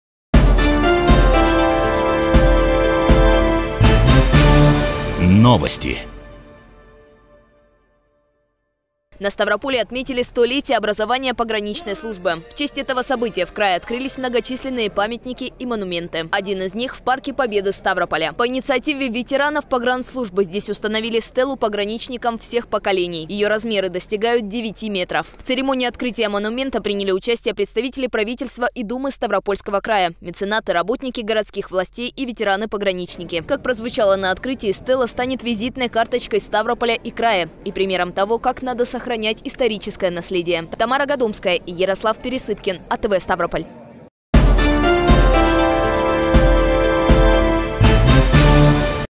Стела пограничникам появилась в Ставрополе
На Ставрополье отметили 100-летие образования Пограничной службы. В честь этого события в крае открылись многочисленные памятники и монументы. Один из них - в Парке Победы Ставрополя.